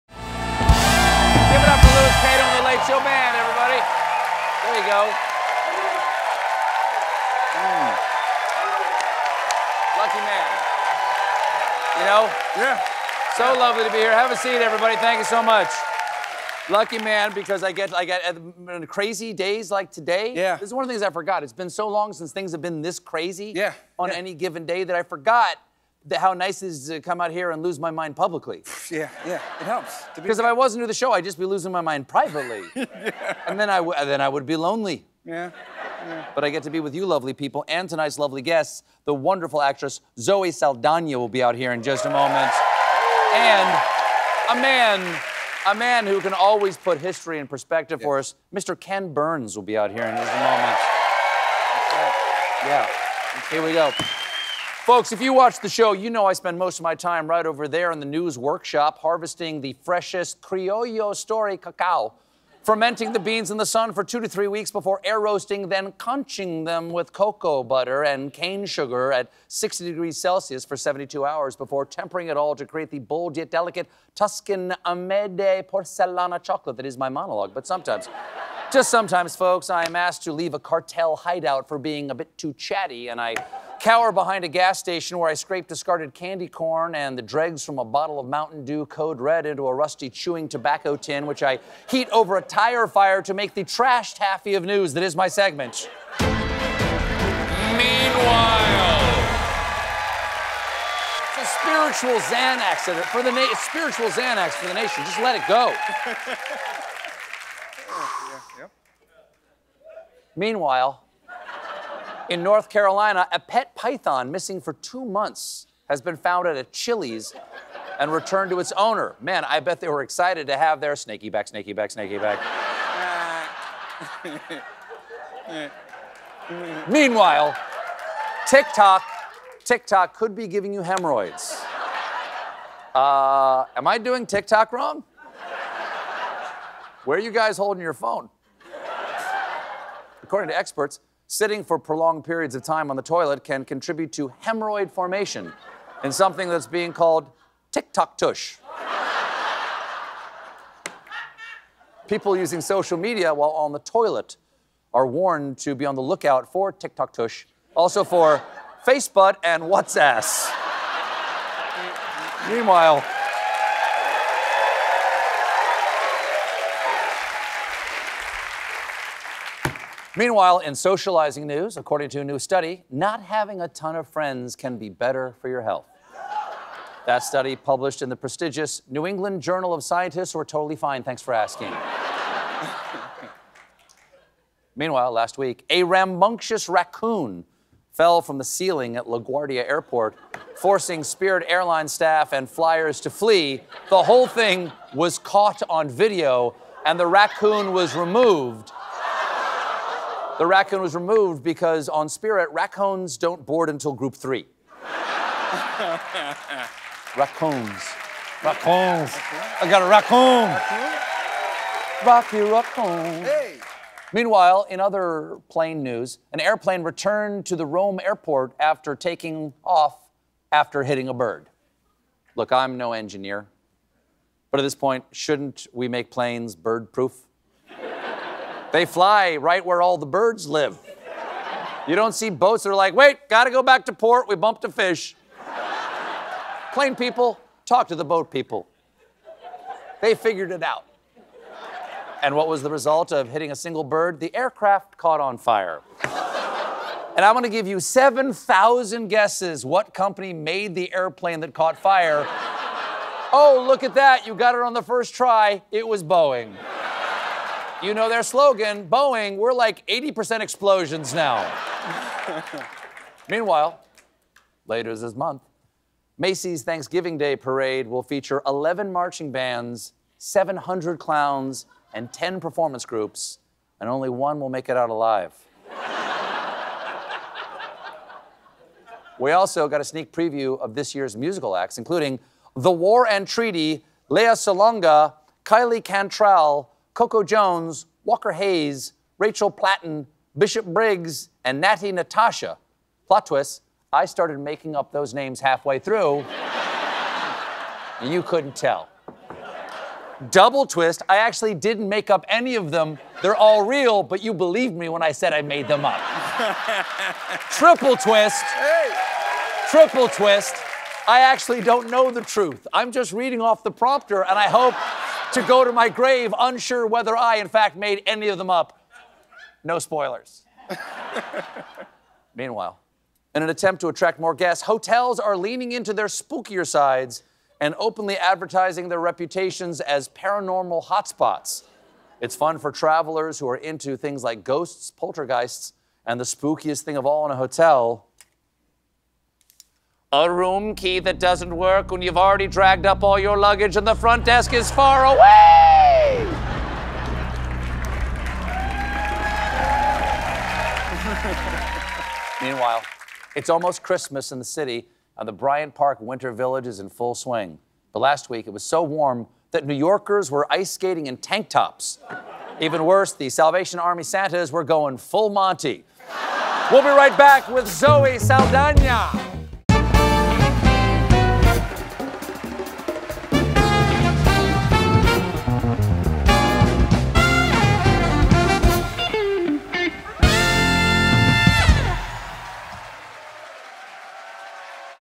Meanwhile... TikTok Tush | LaGuardia Airport Raccoon | Paranormal Hotels
Meanwhile... Don't watch so much TikTok on the toilet, a raccoon fell from the ceiling at New York's LaGuardia airport, and haunted hotels are marketing themselves to travelers who like spooky accommodations.